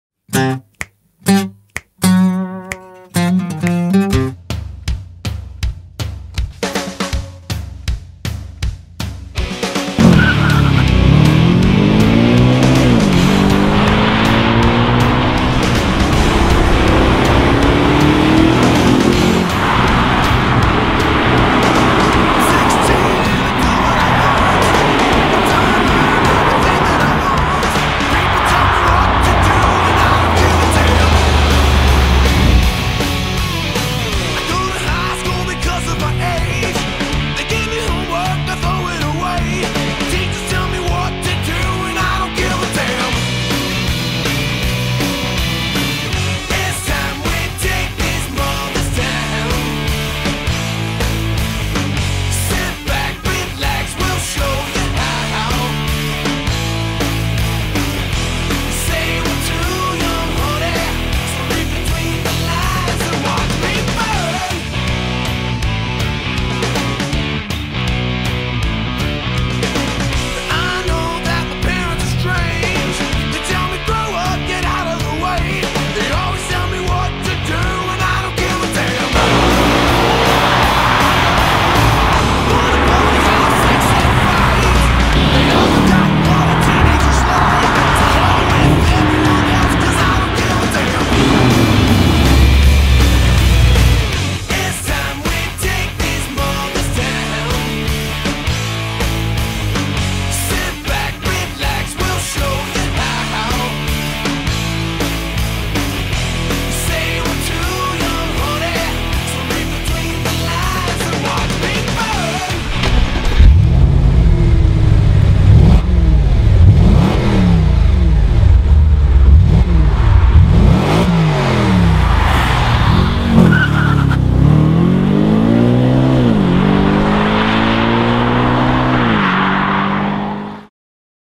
Provkörning Audi RS
Audi RS5 är en riktig sleeper som man inte ska ta för givet. Här finns gott om pulver under huven och bilen skäms verkligen inte för sig på banträffen. Sugis-V8:a, 450 hästar, monsterhärligt avgasljud. Mycket mer behöver man inte.